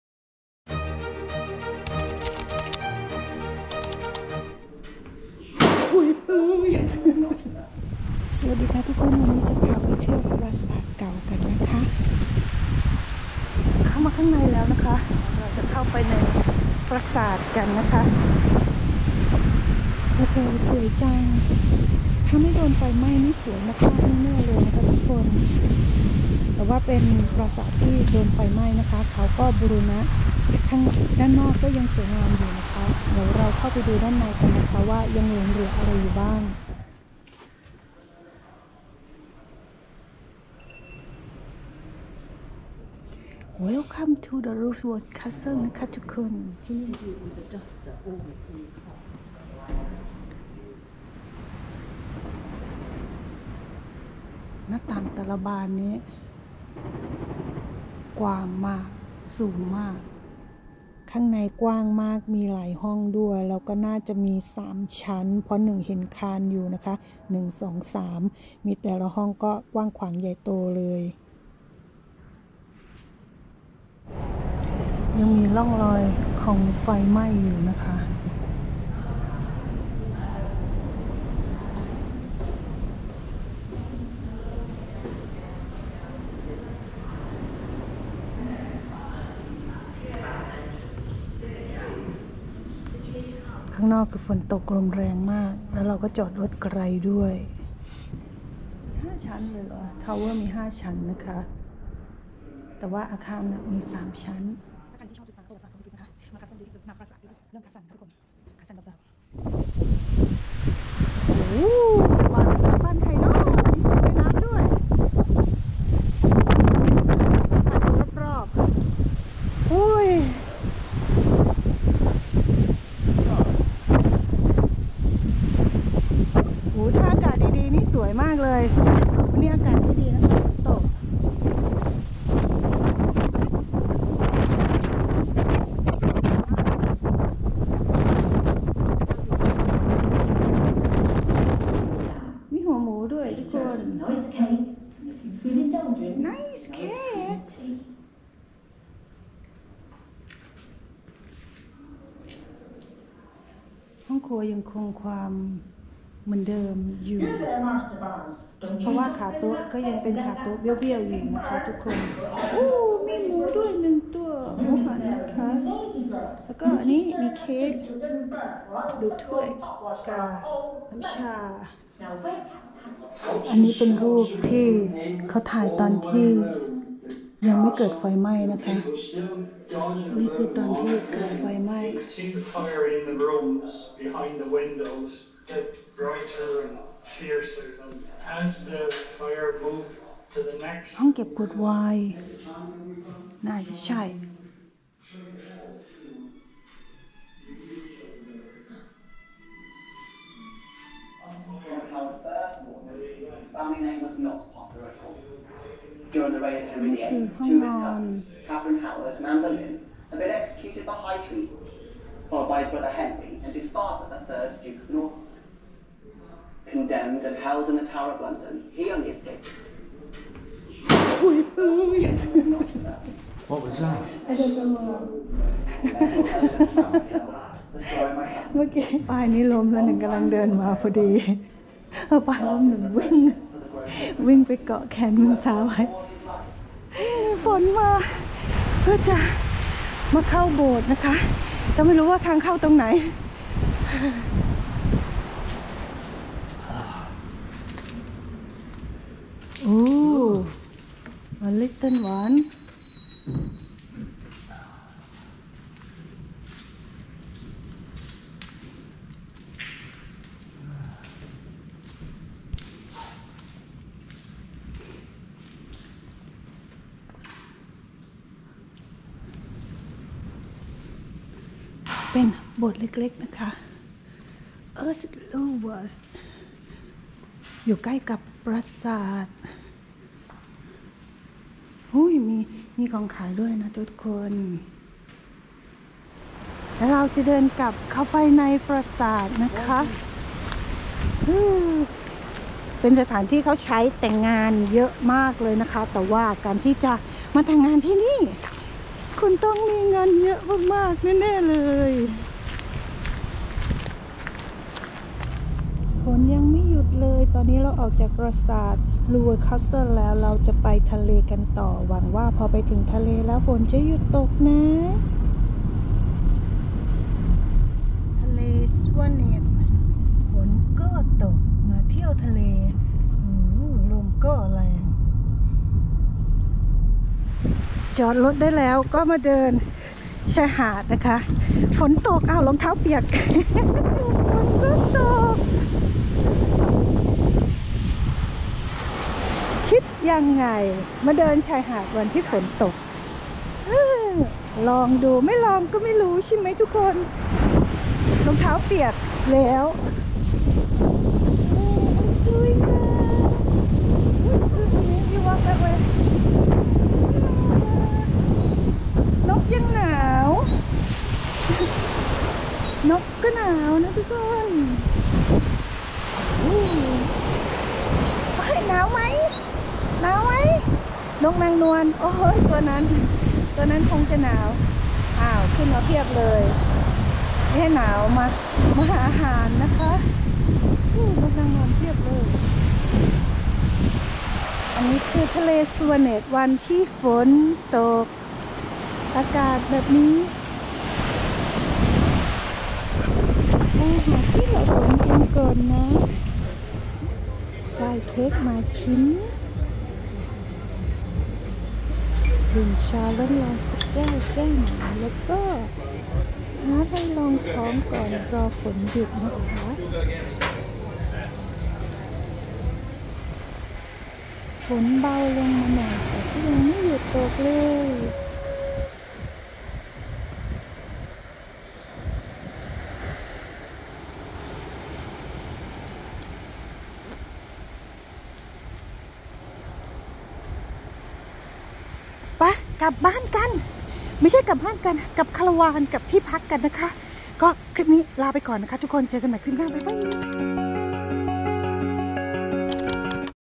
Lulworth Castle ปราสาทเก่าที่โดนไฟไหม้ อะไรทำให้ต้องวิ่ง 4×100ในปราสาท เที่ยวทะเลในวันฝนตก
วันที่ไปคือวันครบรอบแต่งงานค่ะมีเรื่องให้ตื่นเต้นตลอด เป็นสีสันของการเที่ยวมากๆ และขอขอบคุณเพื่อนๆพี่ๆน้องๆทุกคนที่ยังเป็นกำลังใจให้เสมอมานะคะ🙏💕💕